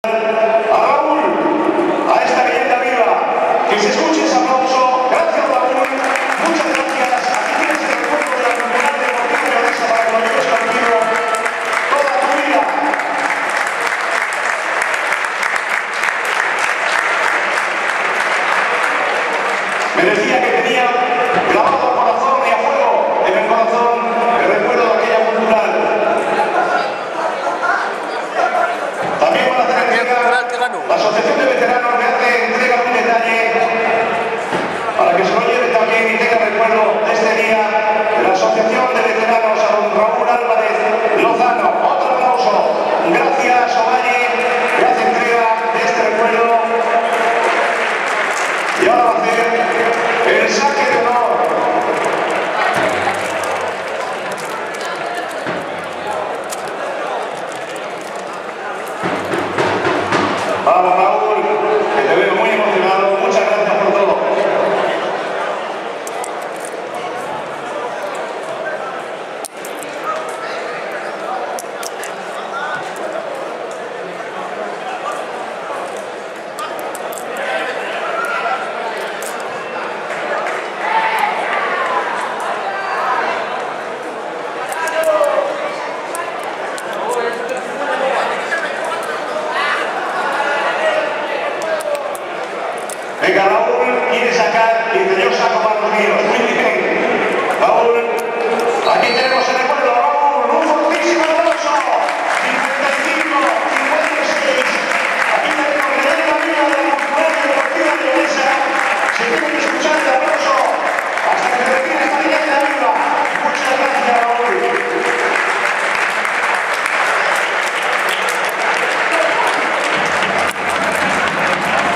Saque de honor de Raúl Álvarez. Temporada 16-17
Saque de honor de Raúl Álvarez, mítico jugador de la plantilla que jugó en Primera División en la temporada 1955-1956.